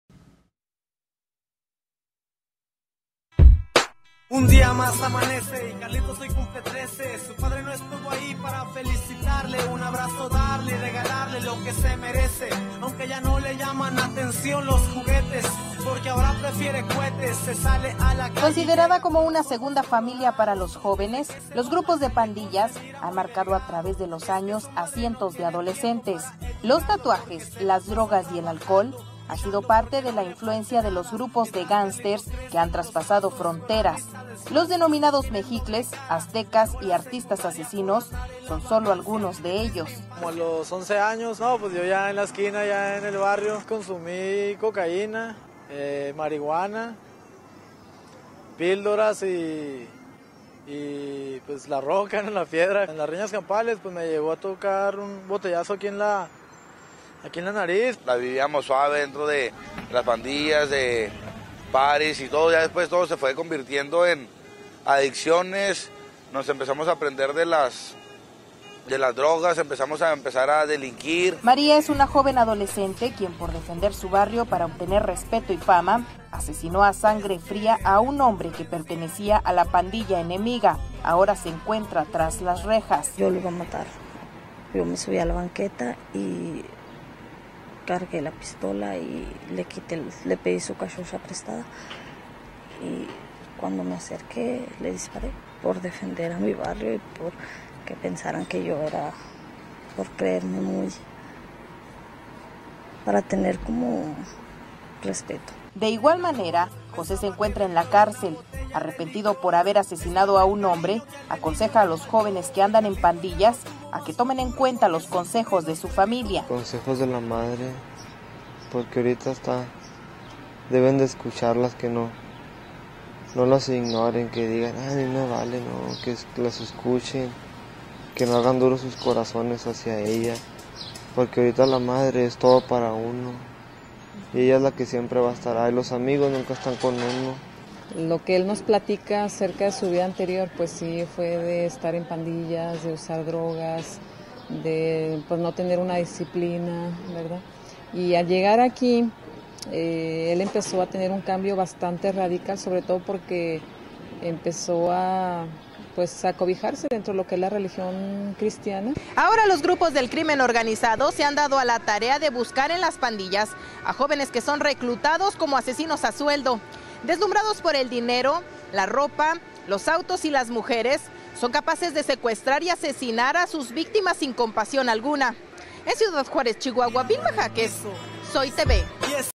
Pandillas en Ciudad Juárez Opiniario por SOiTV
Considerada como una segunda familia para los jóvenes, los grupos de pandillas han marcado a través de los años a cientos de adolescentes. Los tatuajes, las drogas y el alcohol han sido parte de la influencia de los grupos de mafiosos que han traspasado fronteras, los denominados Mexicles, Aztecas y Artistas Asesinos son sólo algunos de ellos.Ahora los grupos del crimen organizado se han dado a la tarea de buscar entre las pandillas a jóvenes que son reclutados como asesinos a sueldo, quienes deslumbrados con dinero, ropa, autos y mujeres son capaces de secuestrar y ejecutar a sus victimas sin compasión alguna.